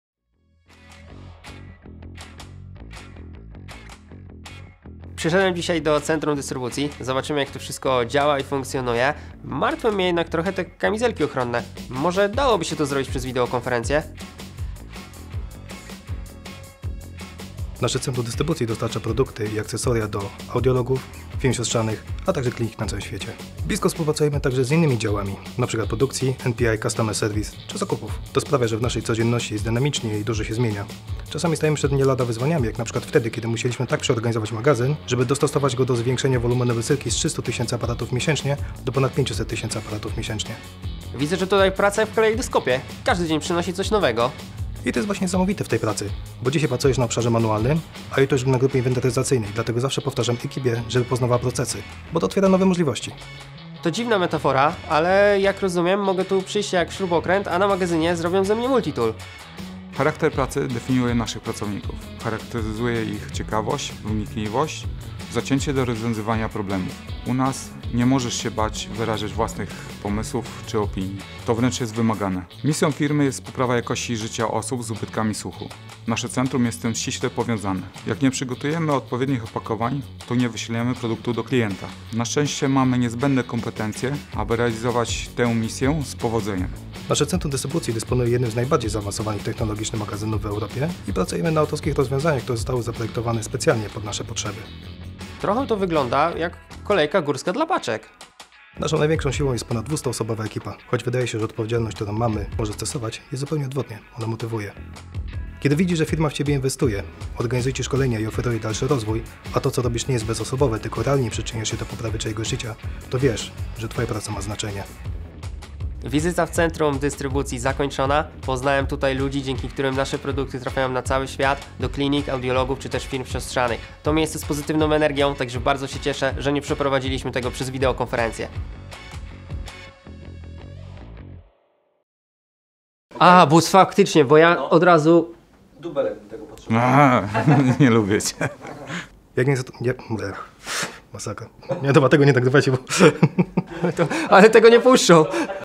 Centrum Dystrybucji
Poznaj Centrum Dystrybucji. Sprawdź nasz typowy dzień w Demant. Dowiedz się więcej o swoich możliwościach rozwoju w naszej firmie. Pracuj z nami w skandynawskim stylu!